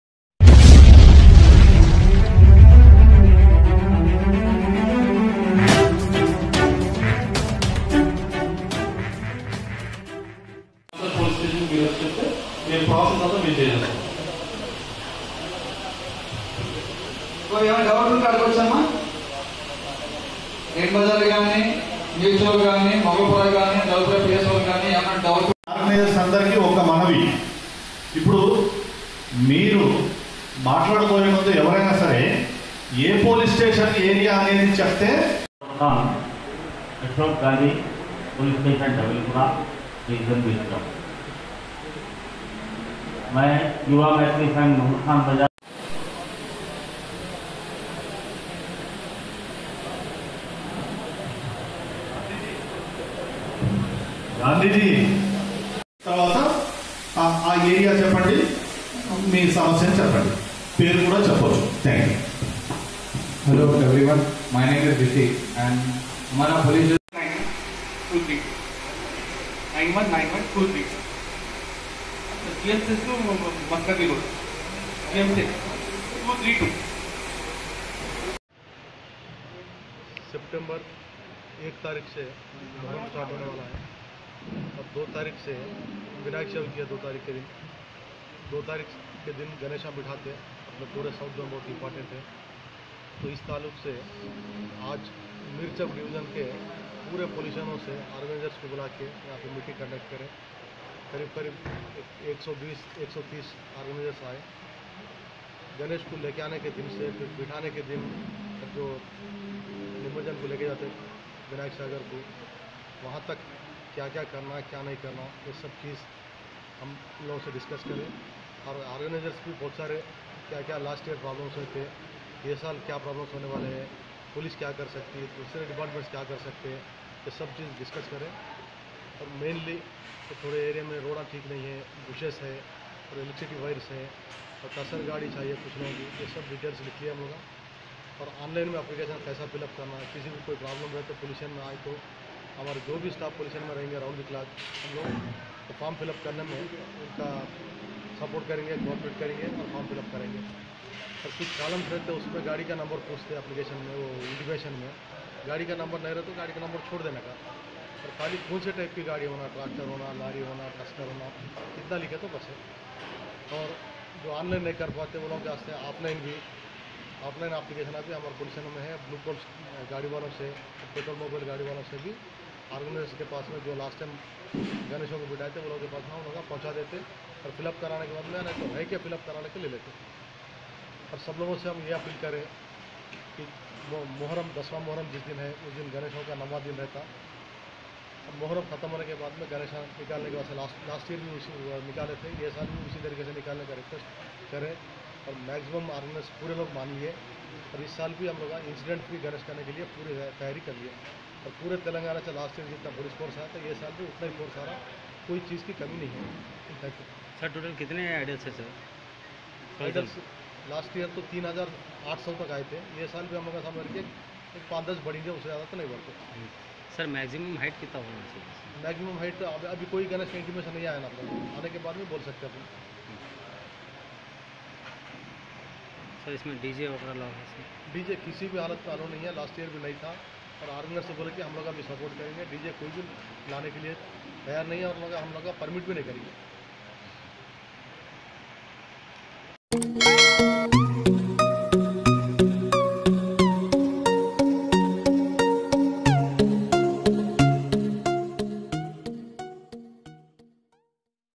SOUTH ZONE ADL DCP HELD MEETING ON UPCOMING GANESH CHATURTI ARRANGEMENTS | @ RIGHT MEDIA NEWS
SOUTH ZONE ADDITIONAL DCP SYED RAFIQ UDDIN HELD MEETING ON UPCOMING GANESH CHATURTI ARRANGEMENTS.

MIR CHOWK POLICE OFFICALS & GANESH PANDALS ORGANISERS DISCUSS THE DETAILS.
MEETING STARTED AT AROUNG 3.30 PM AT ROYAL CLASSIC CONVENTION, ETEBAR CHOWK, HYD..!!

| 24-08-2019 || *REPORT BY : MOHAMMED SHAHBAZ  ||CHIEF JOURNALIST OF *RIGHT_MEDIA_NEWS||.*